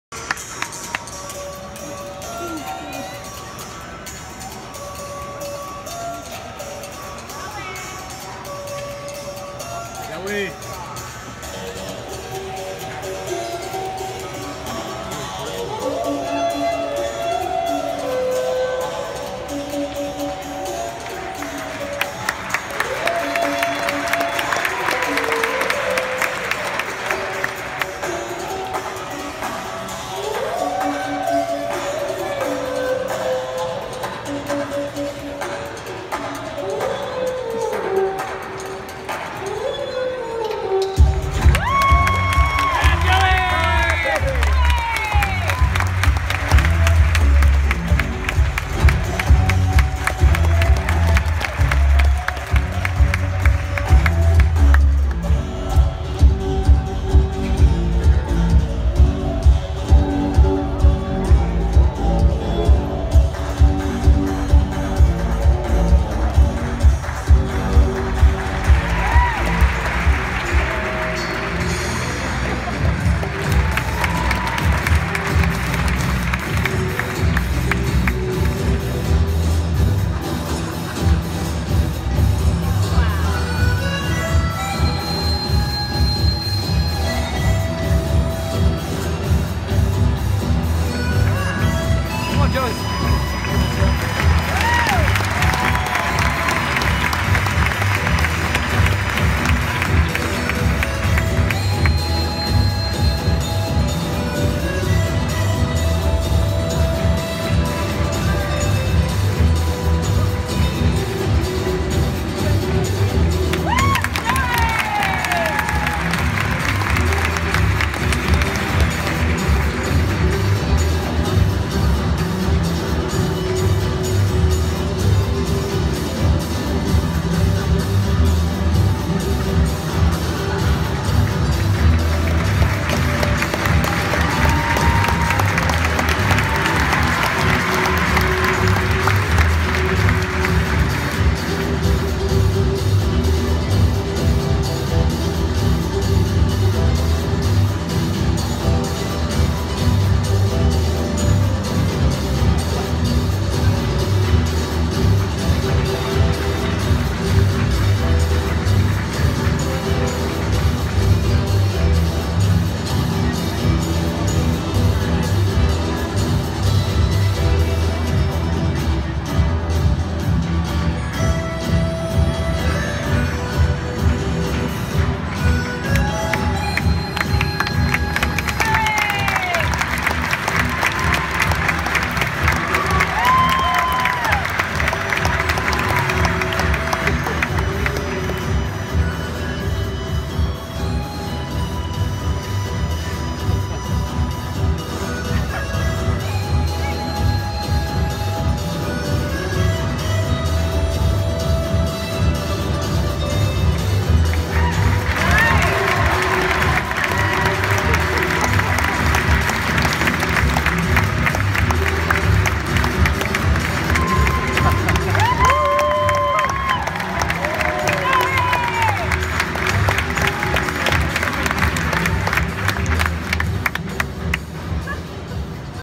Joey Musselman Juggling Flying High Circus April 9, 2016 at Florida State University
Joey Musselman, tall kid, no hat, makes his circus debut in the Flying High Circus